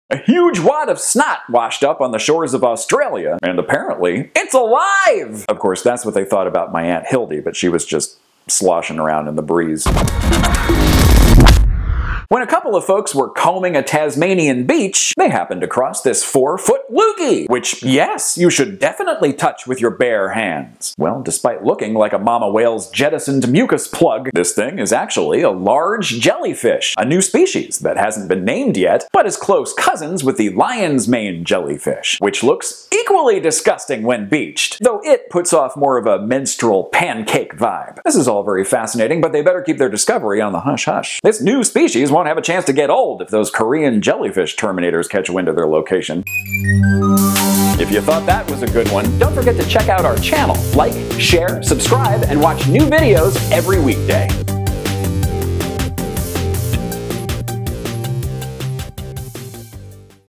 Massive 'Snot Fish' Washes Up in Australia
A huge pool of snot washed up on the shores of Tasmania, and it's aliiiiiiive!

New videos from Mackinley every weekday!